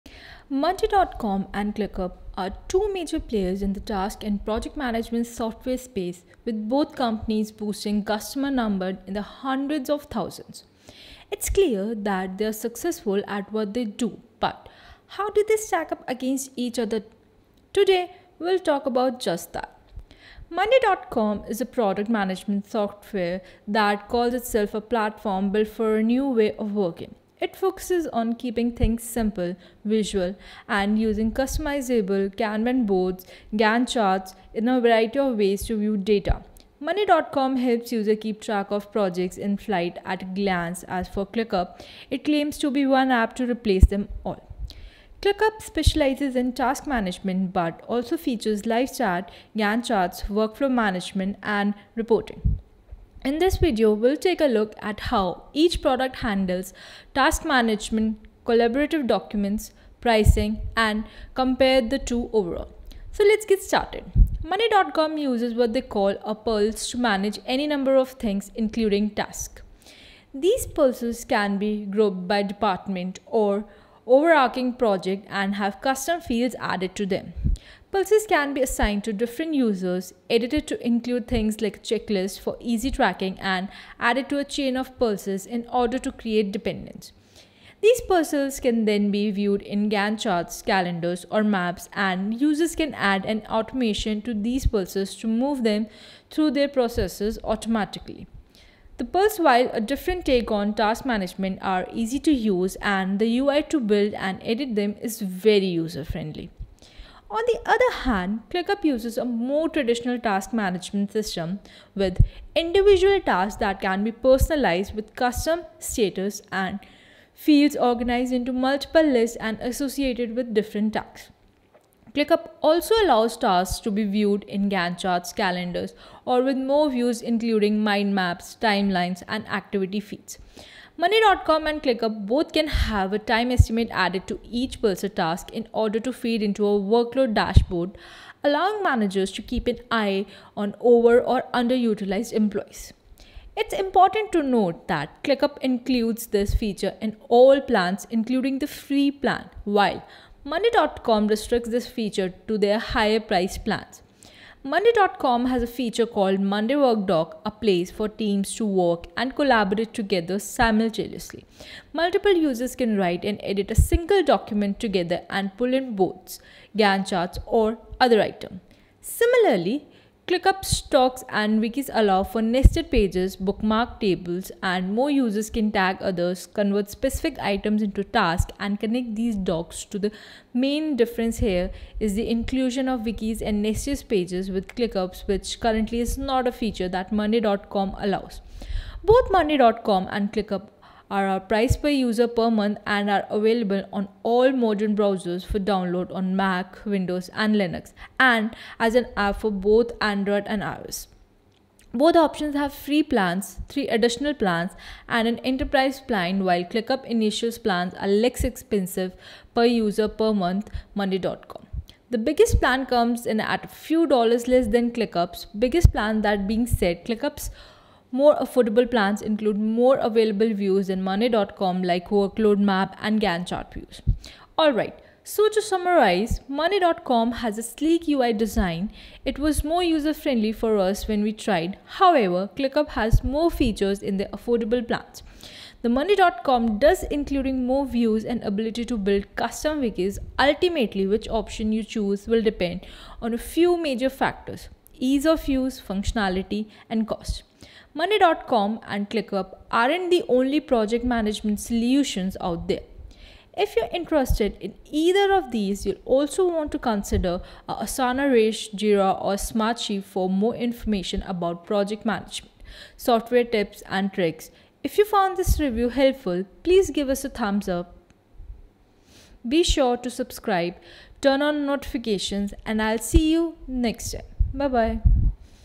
Monday.com VS Clickup for Project Management
In this video I show you a comparison of monday.com vs clickup for project management. So which project management software is better? Monday.com or clickup? Find out in this comparison video!